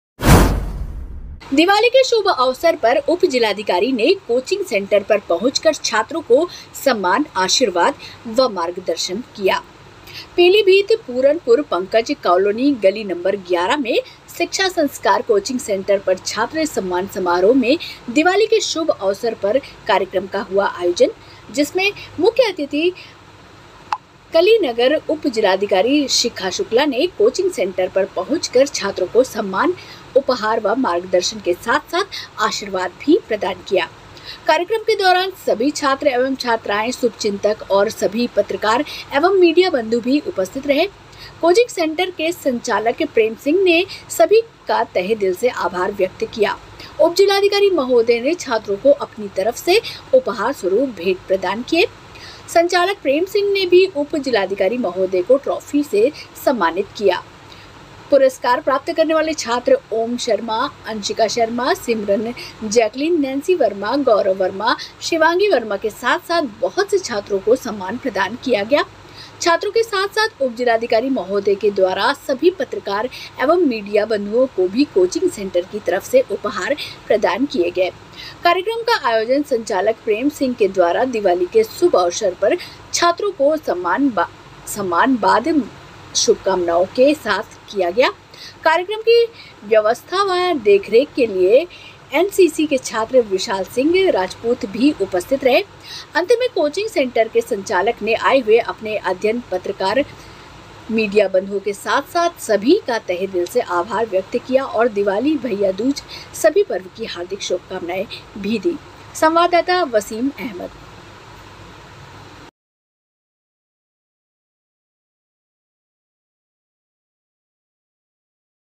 उपजिलाधिकारी ने कोचिंग सेंटर पर पहुंचकर छात्रों को सम्मान आशीर्वाद व मार्गदर्शन प्रदान किया
दिवाली के शुभ अवसर पर उपजिलाधिकारी  ने कोचिंग सेंटर पर पहुंचकर छात्रों को सम्मान आशीर्वाद व मार्गदर्शन प्रदान किया

 पीलीभीत–पूरनपुर पंकज कॉलोनी गली नंबर 11 में शिक्षा संस्कार कोचिंग सेंटर पर छात्र सम्मान समारोह में दिवाली के शुभ अवसर पर कार्यक्रम का हुआ आयोजन जिसमें मुख्य अतिथि कलीनगर उपजिलाधिकारी  शिखा शुक्ला ने कोचिंग सेंटर पर पहुंचकर छात्रों को सम्मान उपहार व मार्गदर्शन के साथ-साथ आशीर्वाद भी प्रदान किया।   कार्यक्रम के दौरान सभी छात्र एवं छात्राएं, शुभचिंतक और सभी पत्रकार एवं मीडिया बंधु भी उपस्थित रहे। कोचिंग सेंटर के संचालक प्रेम सिंह ने सभी का तहे दिल से आभार व्यक्त किया।
 उपजिलाधिकारी महोदय ने छात्रों को अपनी तरफ से उपहार स्वरूप भेंट प्रदान किए। संचालक प्रेम सिंह ने भी उप जिलाधिकारी महोदय को ट्रॉफी से सम्मानित किया। पुरस्कार प्राप्त करने वाले छात्र ओम शर्मा, अंशिका मिश्रा, सिमरन, जैकलिन, नैंसी वर्मा, गौरव वर्मा, शिवांगी वर्मा के साथ-साथ बहुत से छात्रों को सम्मान प्रदान किया गया। छात्रों के साथ-साथ उपजिलाधिकारी महोदय के द्वारा सभी पत्रकार एवं मीडिया बंधुओं को भी कोचिंग सेंटर की तरफ से उपहार प्रदान किए गए। कार्यक्रम का आयोजन संचालक प्रेम सिंह के द्वारा दिवाली के शुभ अवसर पर छात्रों को सम्मान बाद शुभकामनाओं के साथ किया गया। कार्यक्रम की व्यवस्था व  देखरेख के लिए एनसीसी के छात्र विशाल सिंह राजपूत भी उपस्थित रहे। अंत में कोचिंग सेंटर के संचालक ने आए हुए अपने अध्ययन पत्रकार मीडिया बंधुओं के साथ साथ सभी का तहे दिल से आभार व्यक्त किया और दिवाली, भैया दूज सभी पर्व की हार्दिक शुभकामनाएं भी दी।

संवाददाता वसीम अहमद